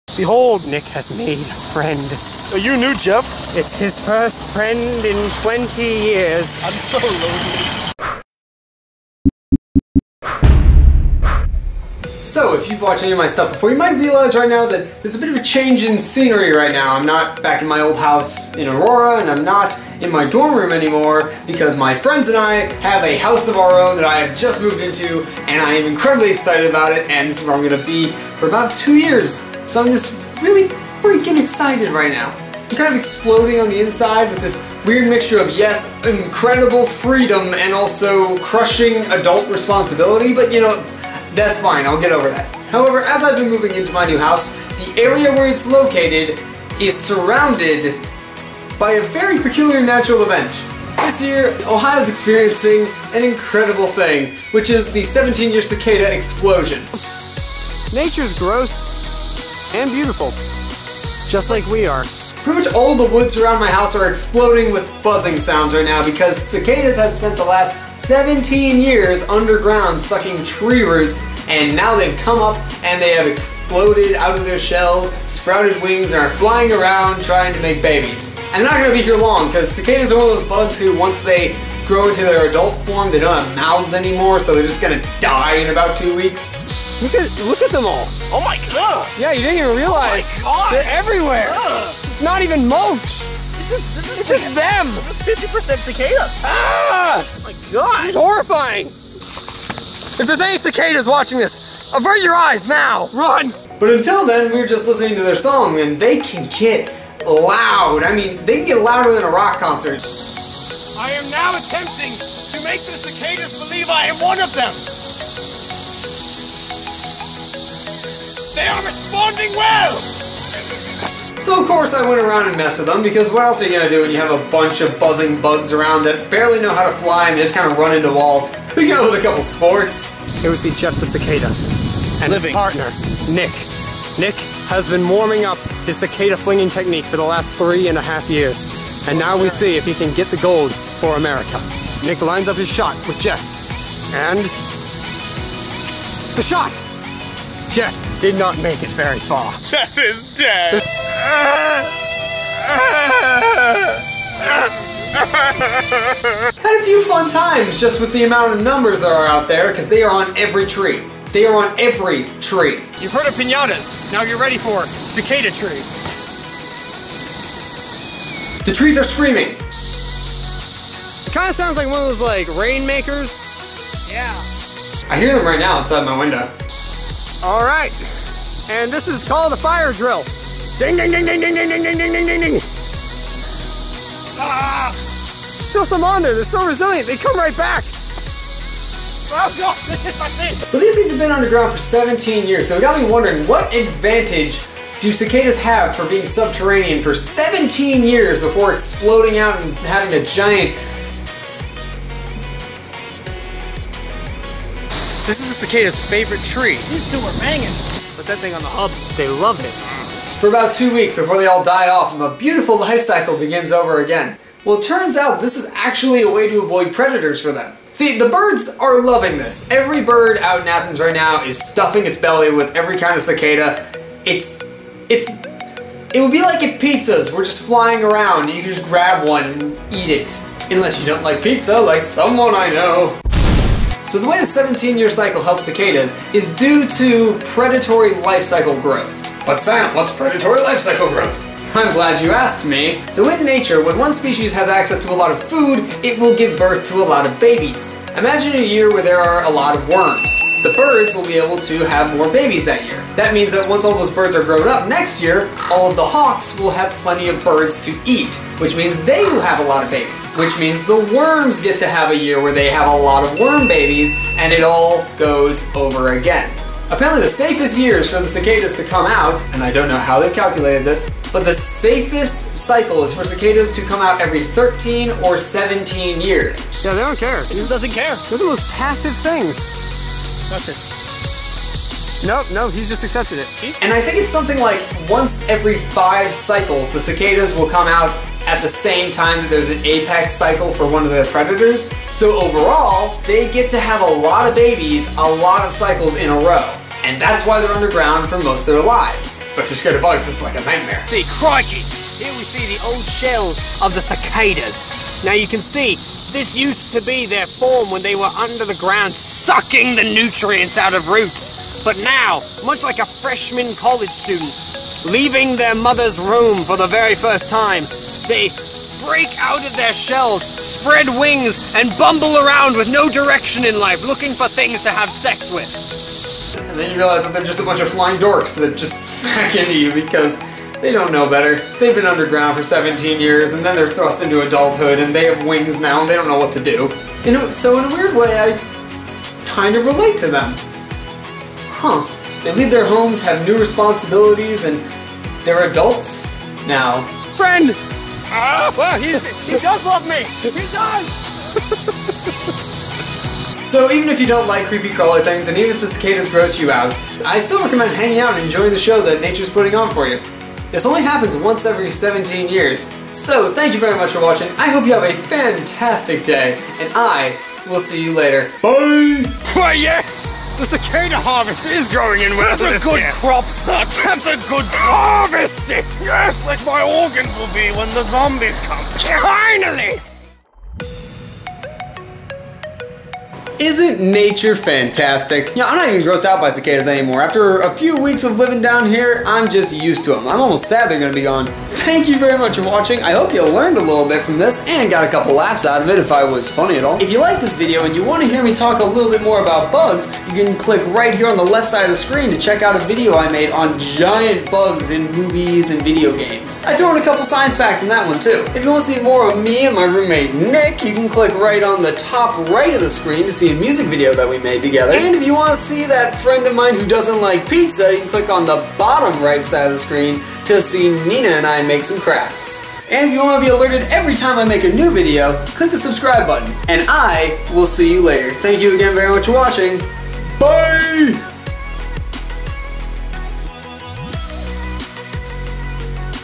17 Year Cicada Explosion!
There are cicadas everywhere! Time to mess with them while also trying to understand them. Man, I should be a diplomat.